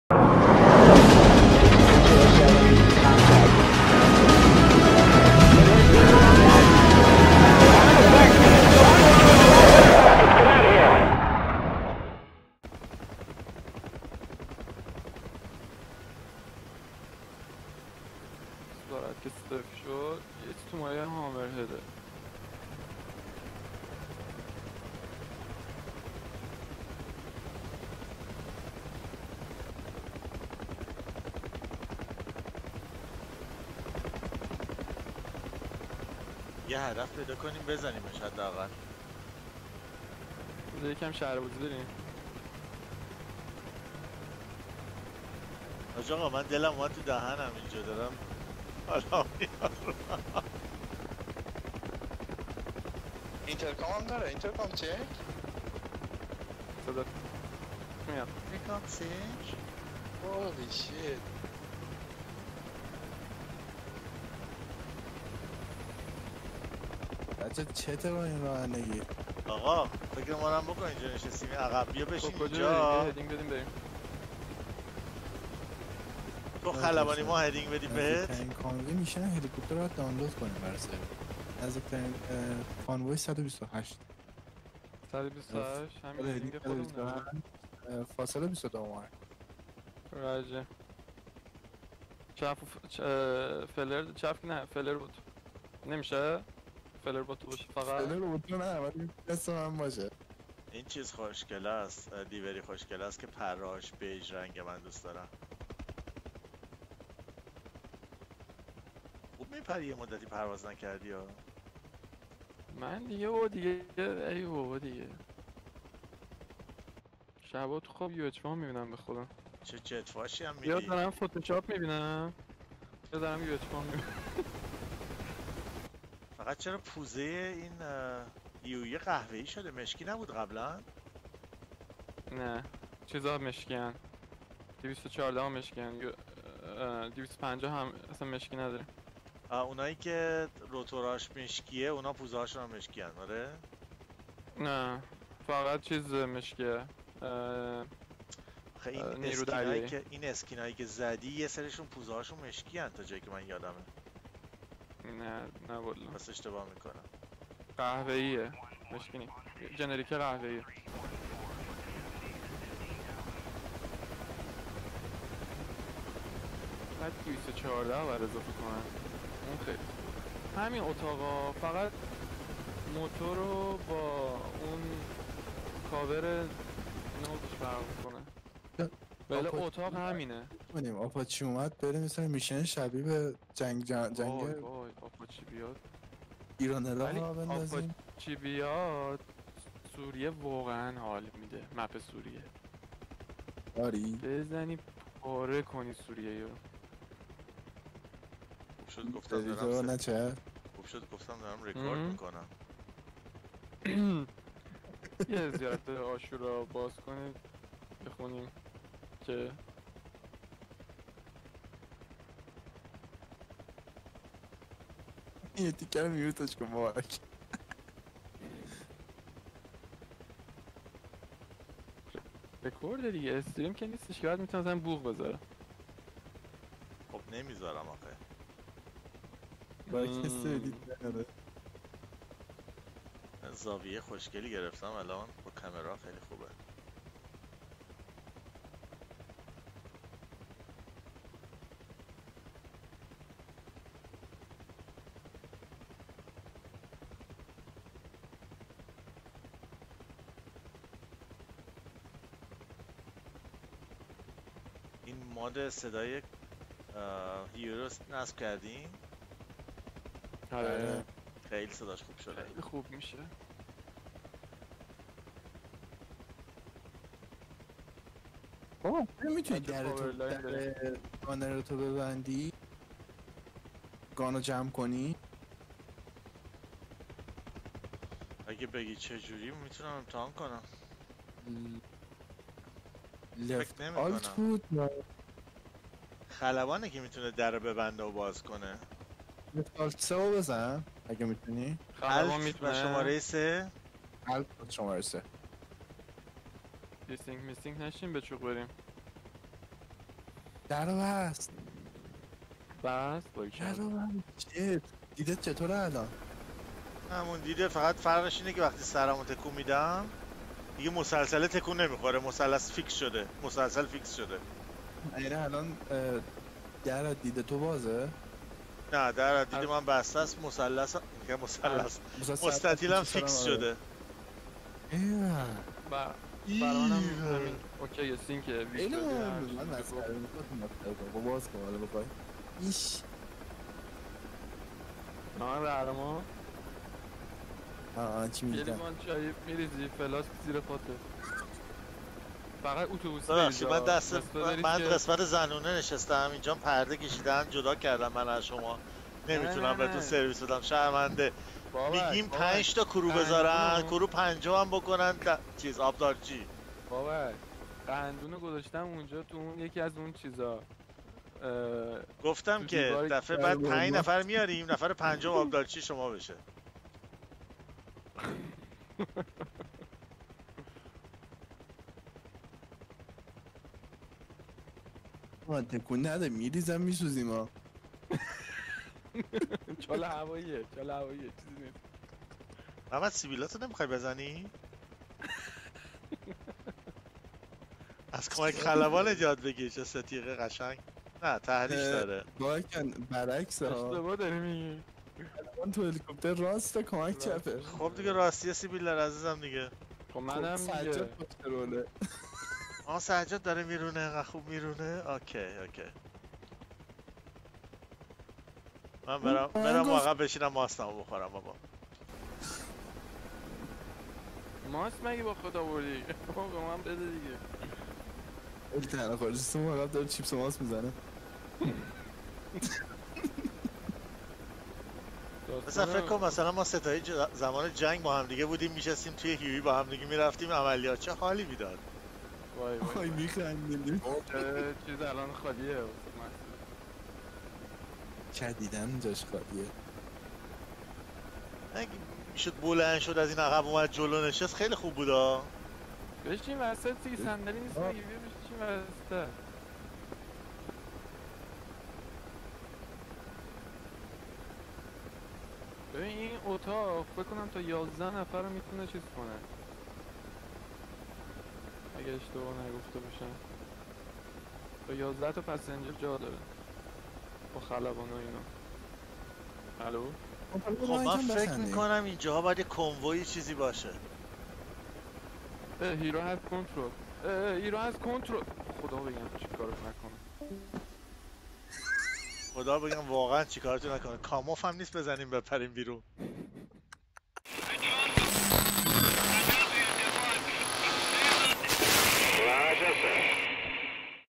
Multi crew UH-1H first impressions
Avalin tajrobeye parvaze chand nafare ba Huey